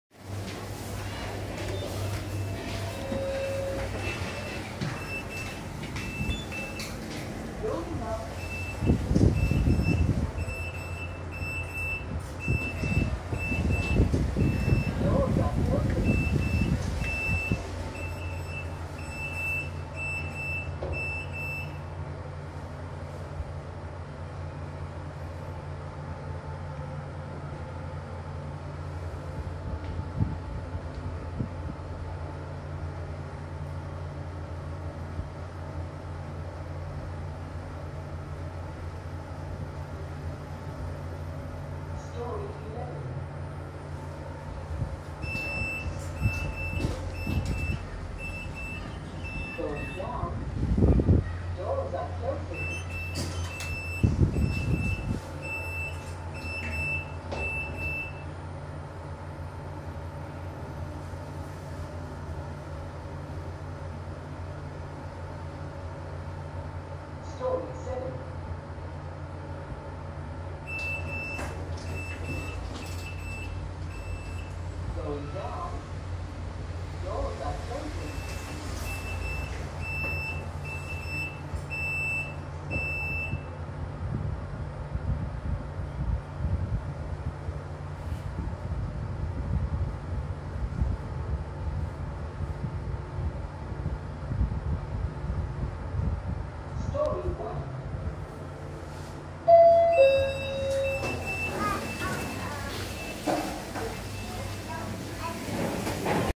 HDB Block 122 Fujitec Ang Mo Kio Lift Ride (New)
This video is to ride a new lift at HDB Block 122 Fujitec Ang Mo Kio. Taken on 6 May 2018.